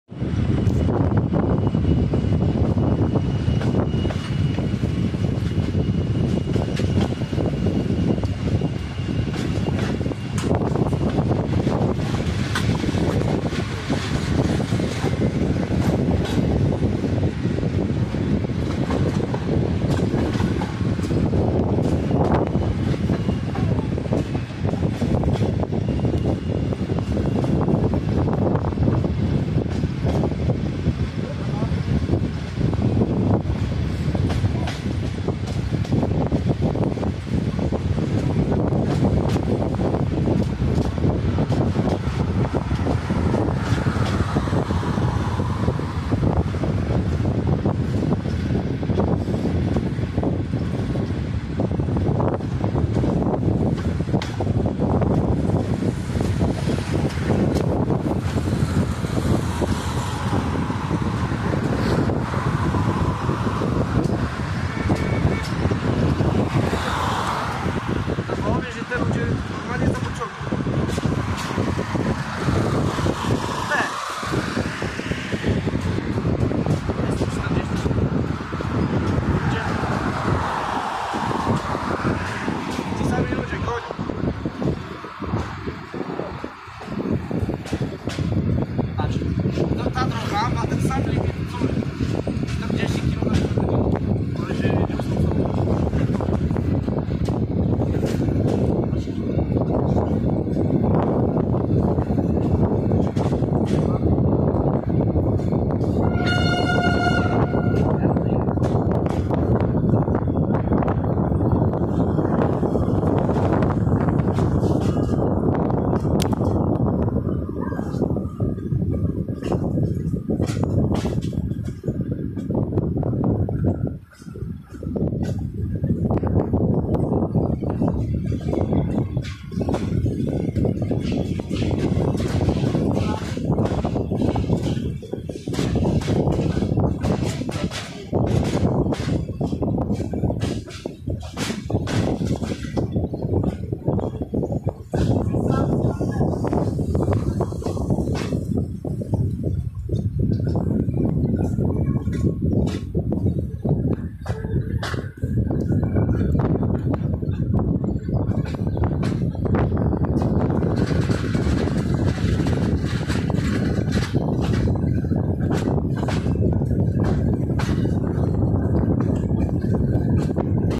Wjazd do Leśnej pociągiem TurKolu "Leśniczy"
Oczywiście film był nagrany w Sobotę 25 Marca podczas wydarzenia "Wiosna 2023"